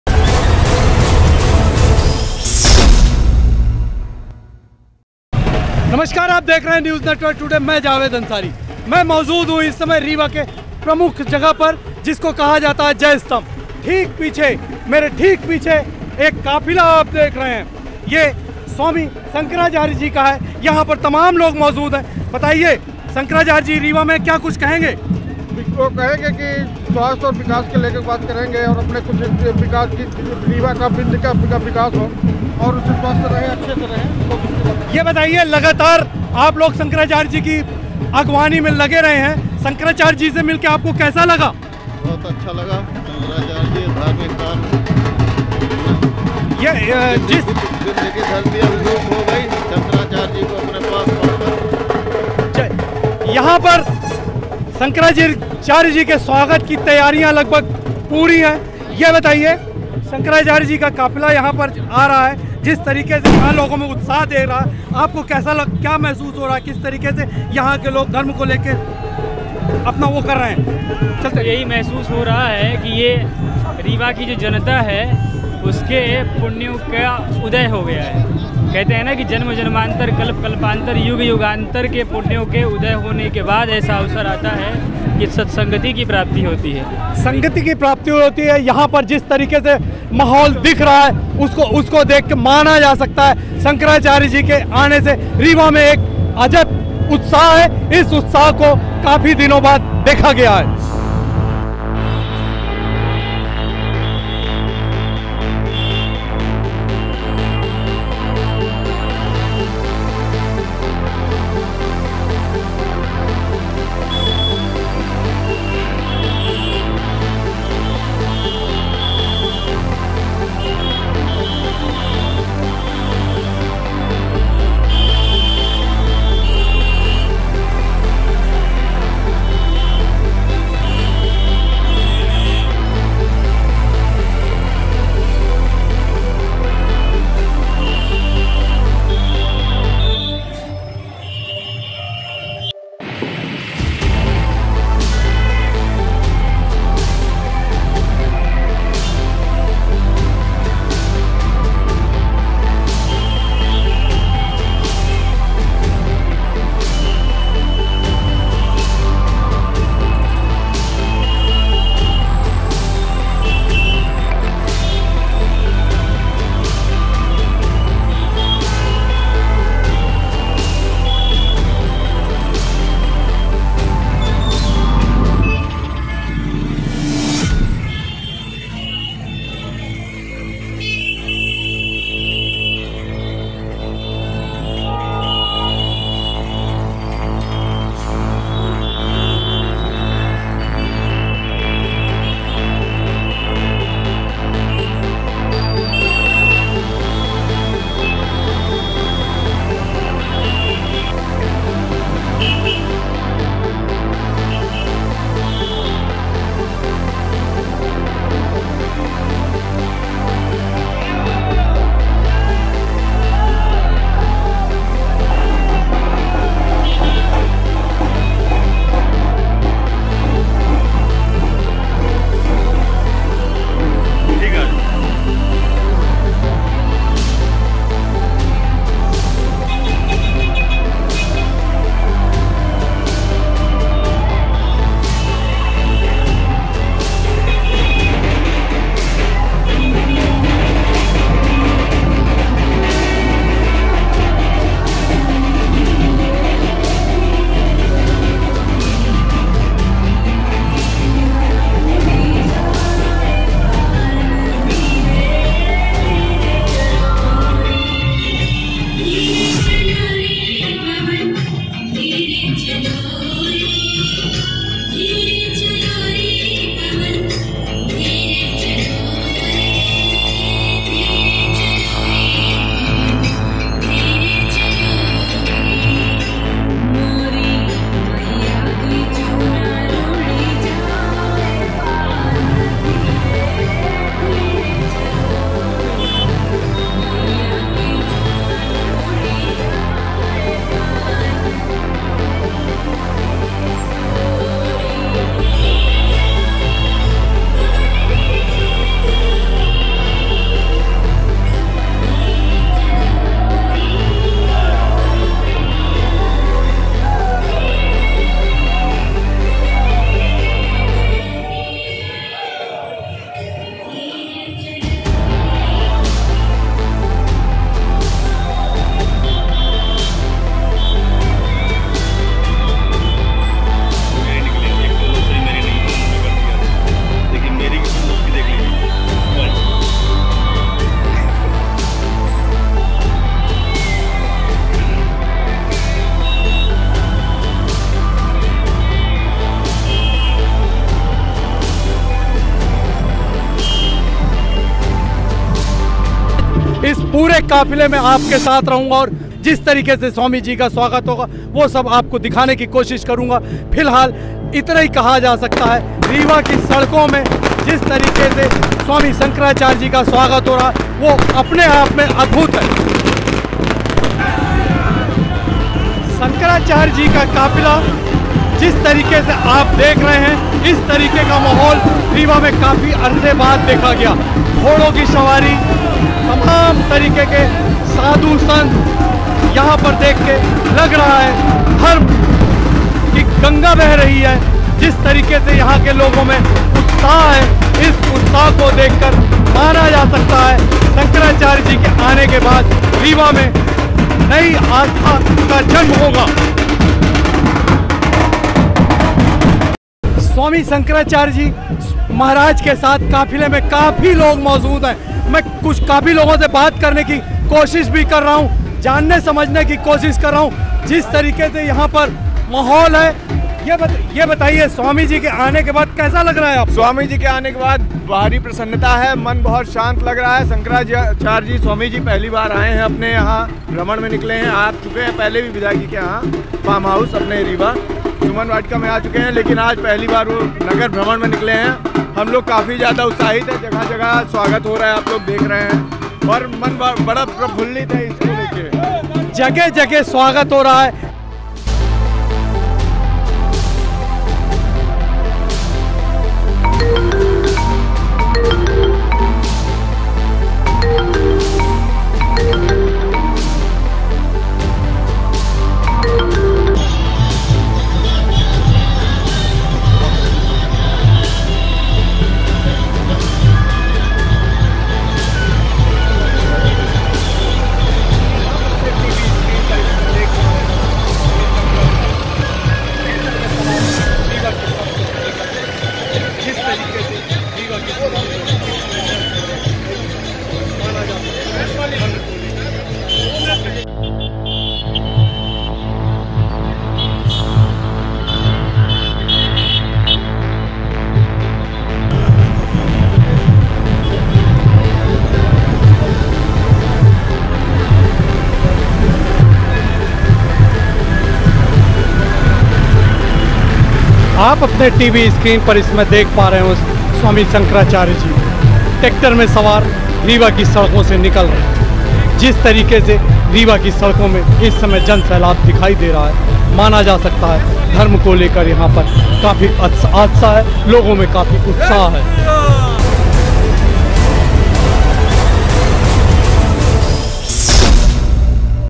शंकराचार्य स्वामी स्वरूपानंद जी महाराज का भव्य स्वागत ||NEWS NETWORK TODAY
शंकराचार्य स्वामी स्वरूपानंद जी महाराज का भव्य स्वागत

द्वारिका बद्रीनाथ पीठ के स्वामी शंकराचार्य स्वामी स्वरूपानंद जी महाराज रीवा की गलियों में रीवा की जनता को आशीर्वाद देने ट्रैक्टर से बने रथ पर सवार होकर निकले